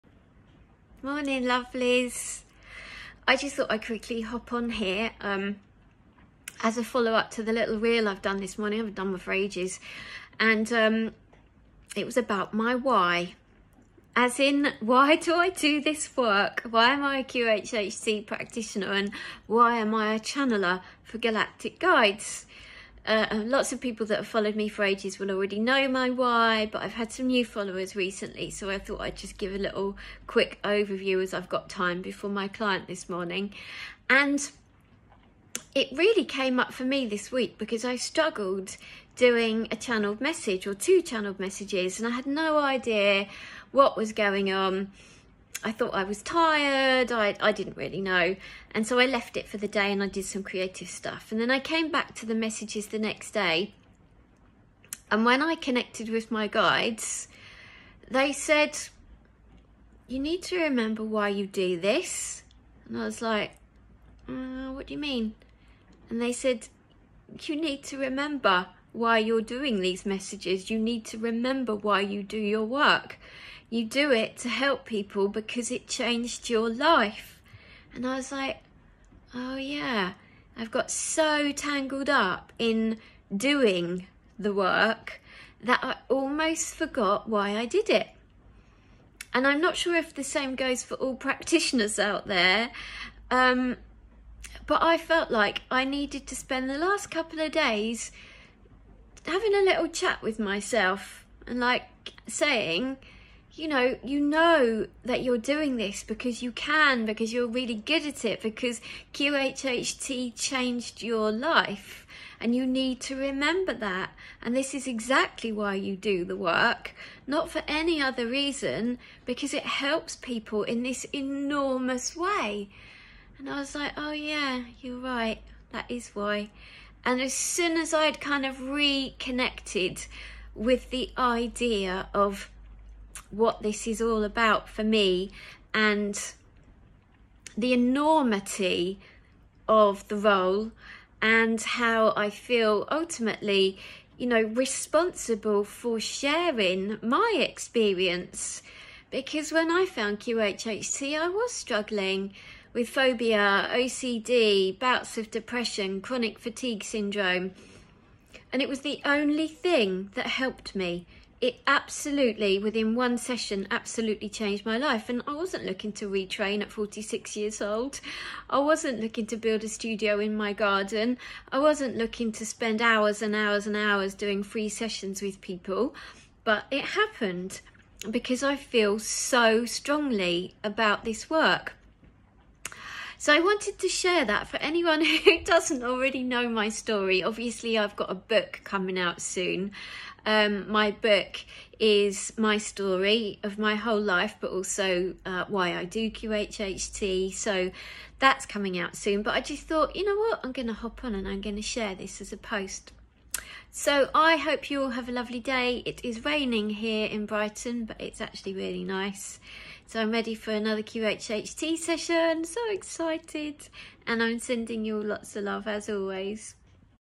𝕄𝕐 𝕎ℍ𝕐!
I wasn’t looking for a new career.

I wasn’t looking to spend time retraining at the age of 46.

I wasn’t looking to open my home to strangers.

But nonetheless, all of that happened!

When I found QHHT I was struggling with phobia, OCD, Chronic fatigue and bouts of depression.

The session that I experienced absolutely CHANGED MY LIFE!

And it's the one and only reason that I practice QHHT today.

Thanks, Universe, for putting me on this path…

I have never felt more settled, more empowered or more full of love than I do now!

If you are looking to change your life beautiful, maybe Quantum Healing Hypnosis Technique could be for you too.

Sending BIG LOVE, Georgina xxx